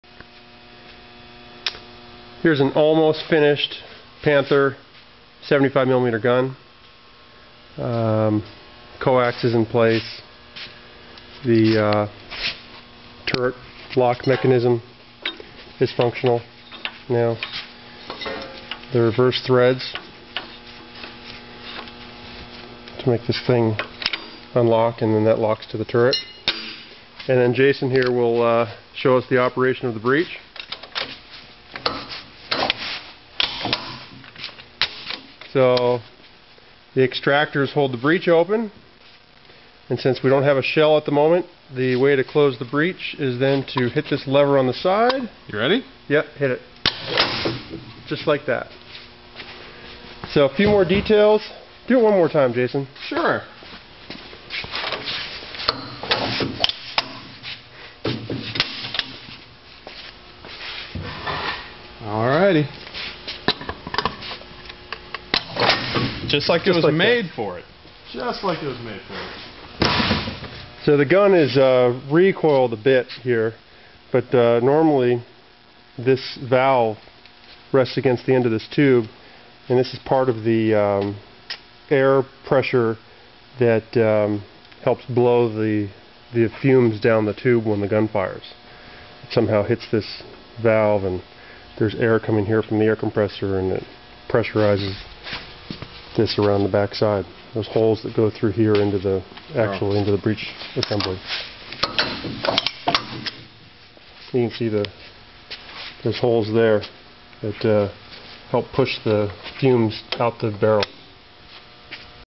Panther Tank 75mm gun tour!
A quick video showing the gun and operating breech mechanism of a Panther tank.  This non- functional gun is part of the collection of the Military Vehicle Technology Foundation.